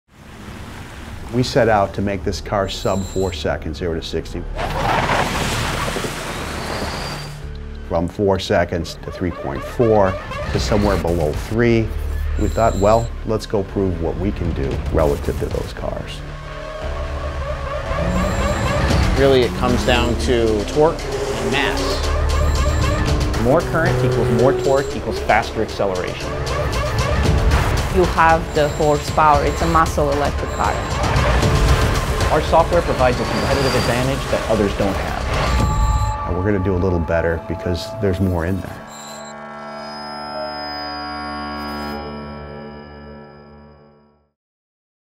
Faraday Future | FF Prototype v. Bentley Bentayga, Ferrari 488 GTB, Tesla Model X P100D
We put our powertrain to the test against a Bentley Bentayga, a Ferrari 488 GTB, and a Tesla Model X P100D Ludicrous.
FirstForward and first across the finish line.
Witness the 0-60 MPH verified results at CES2017.